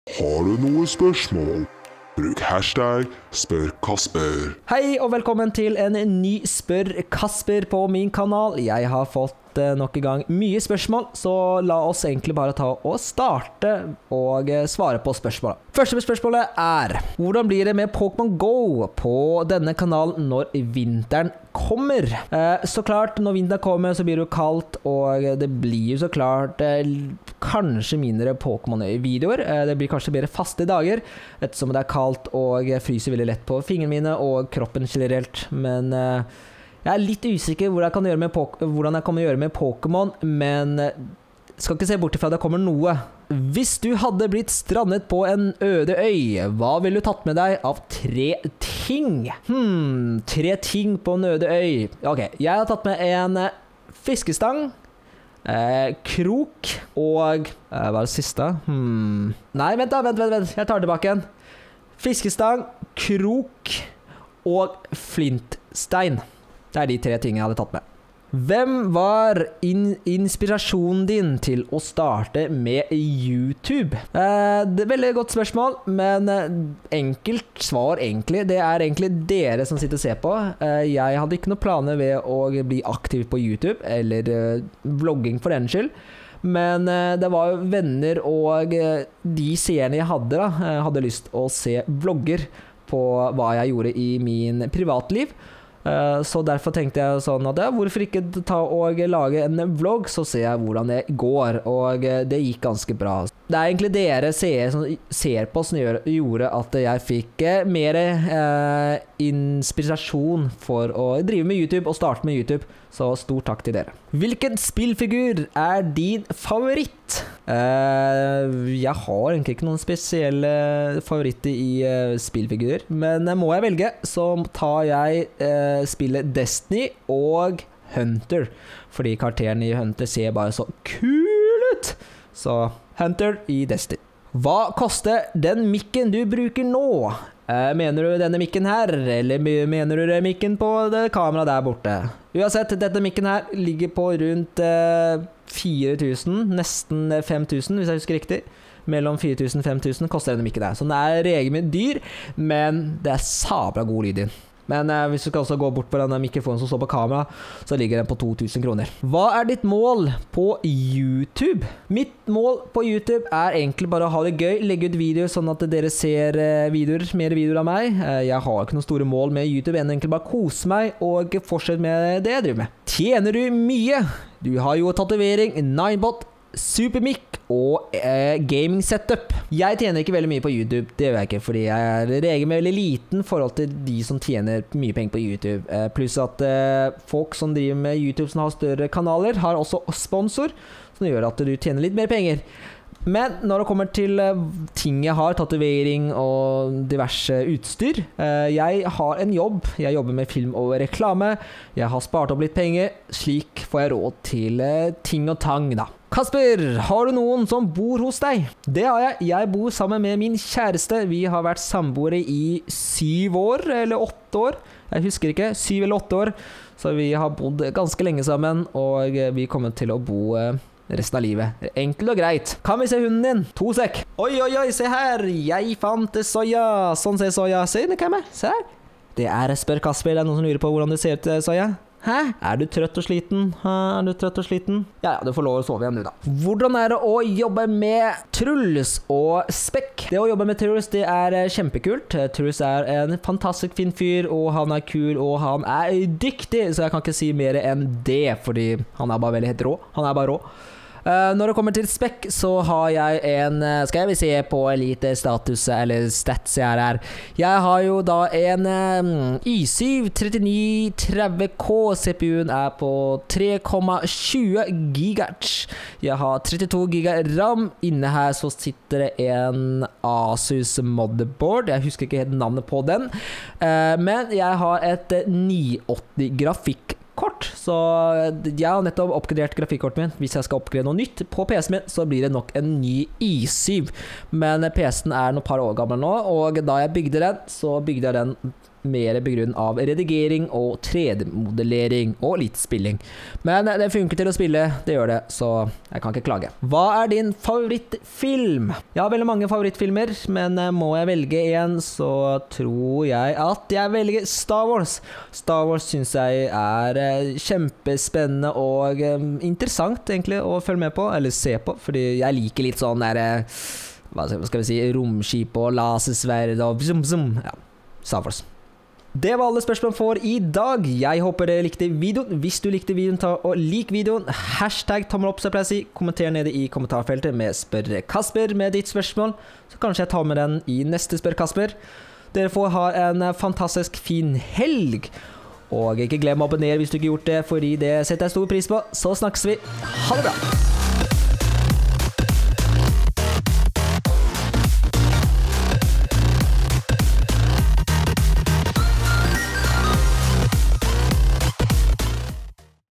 SpørKasper - Kan du vise hunden din(beklager for litt feil i redigeringen)
Hei igjen alle sammen, her er en ny Q&A der jeg svarer på spørsmåler som dere lurer på.

jeg s beklager for at spørsmålene ikke ble langt inn riktig. .

Ønsker du og spørre meg om ting, alt du trenger og gjøre er og skrive SpørKasper og ditt spørsmål